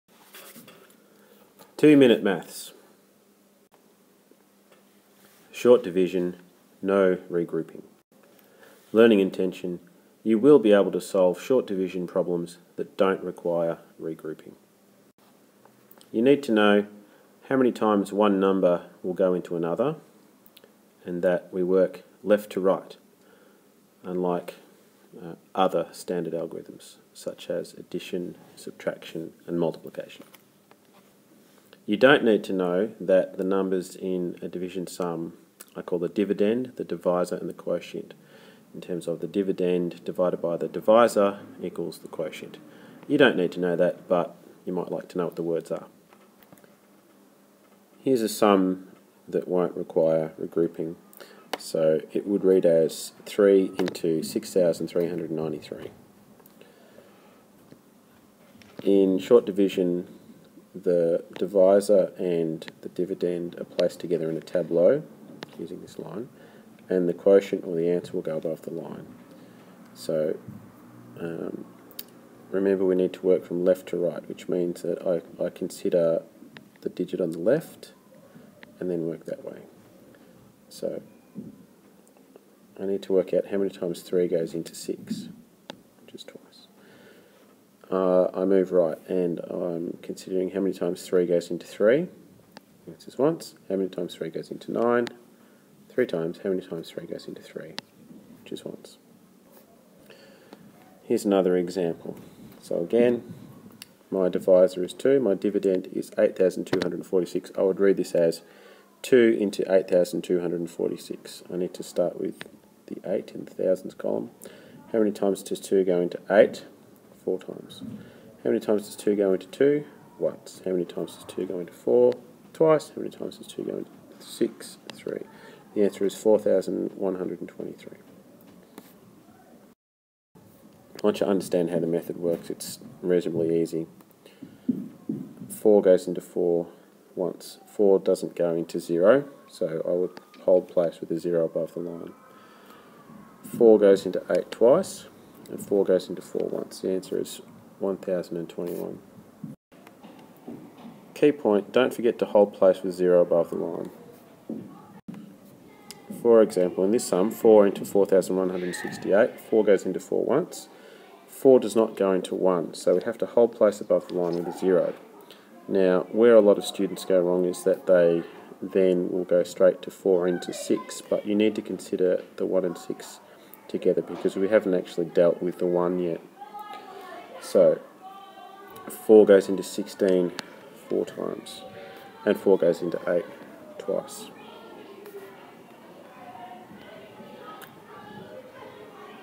How To Do Short Division No Regrouping
Math workbooks for homeschoolers and teachers!

How to perform the short division algorithm on sums that won't require regrouping.